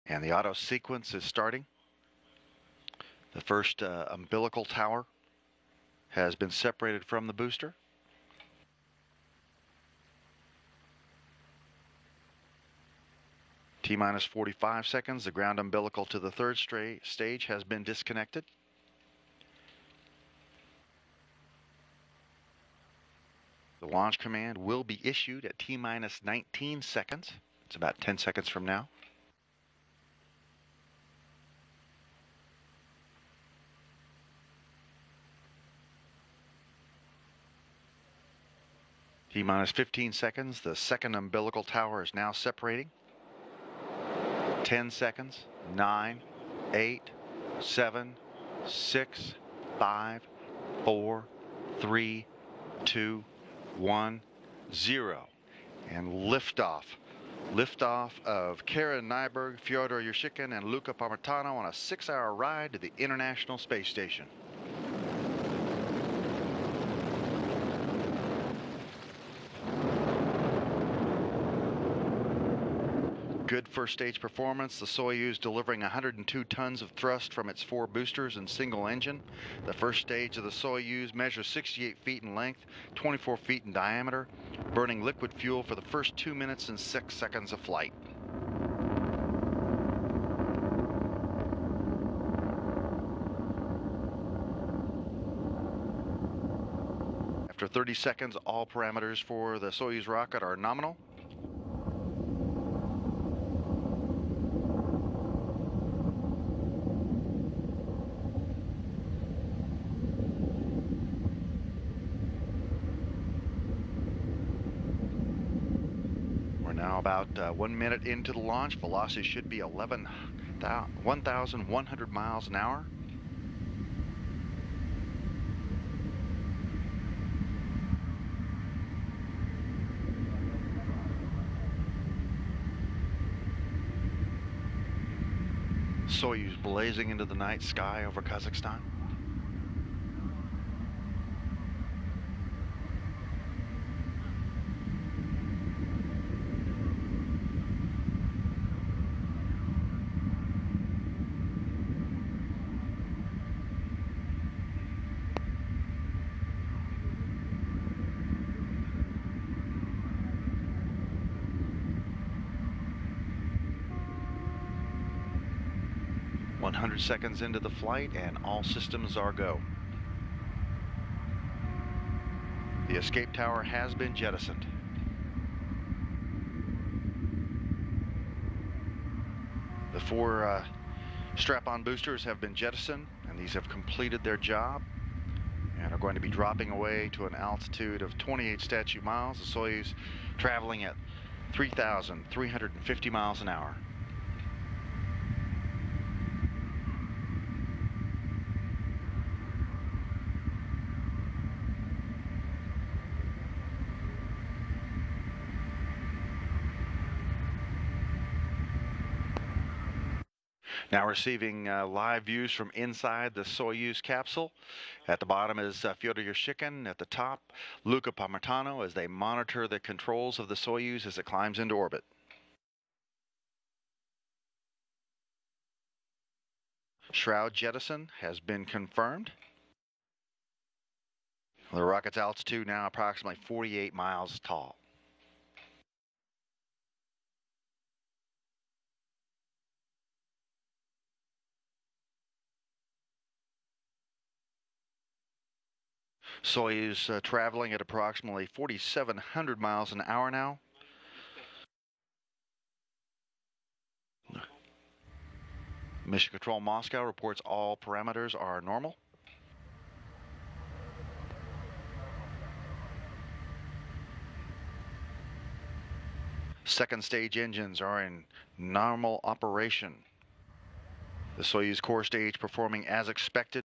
Blast-Off! Six Hour Ride Underway For New Space Station Crew | Video
Soyuz Commander Fyodor Yurchikhin of the Russian Federal Space Agency (Roscosmos), NASA astronaut Karen Nyberg and European Space Agency (ESA) astronaut Luca Parmitano launched to the International Space Station on May 28th, 2013.